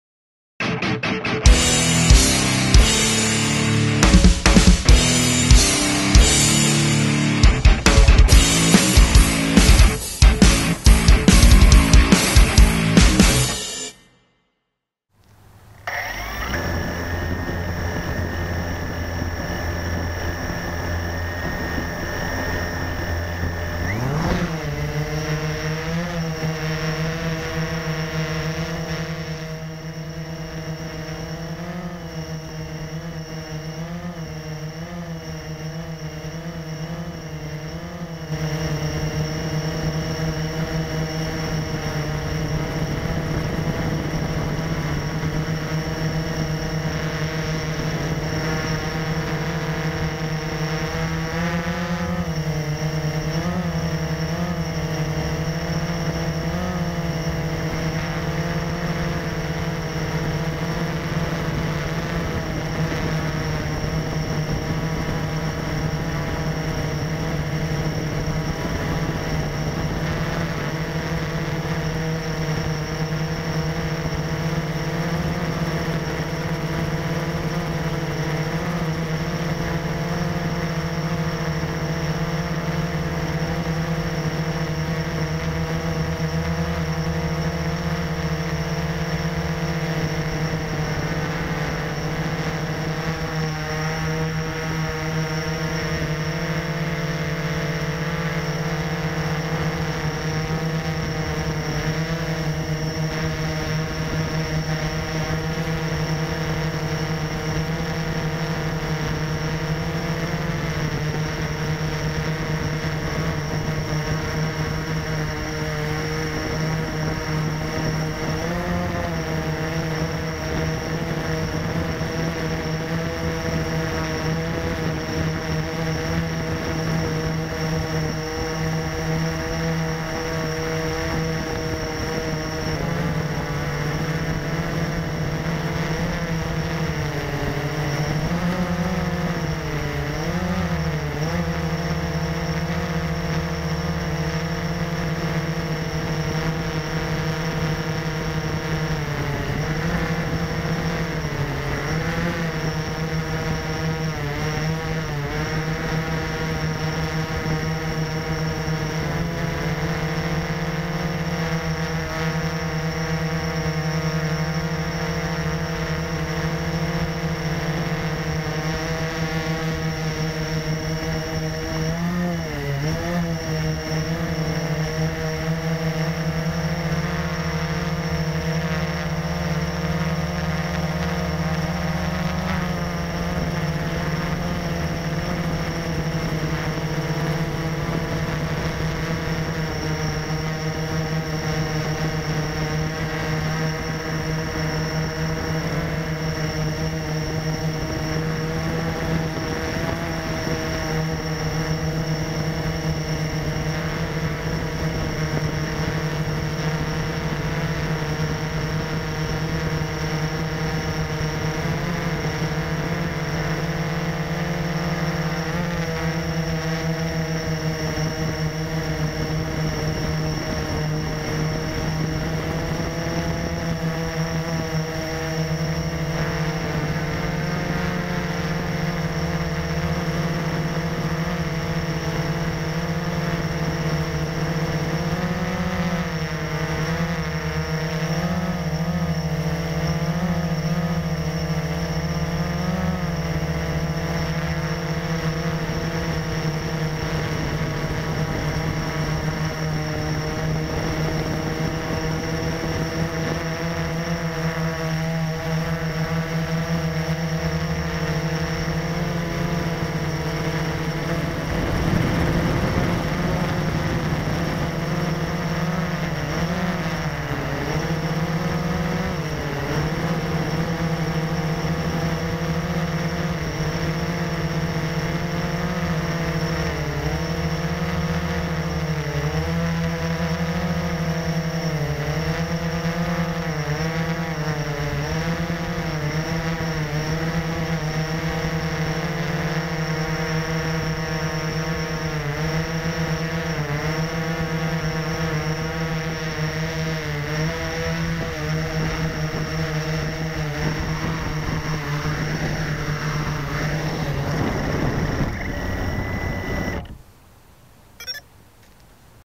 Blade 350 QX - first test flight
I finally got in a short test flight with my new Blade 350 QX. Nothing fancy - mainly hovering over my driveway -  but I'm stoked to have finally gotten it in the air.

Video courtesy of one of my 808 #16 keychain cameras.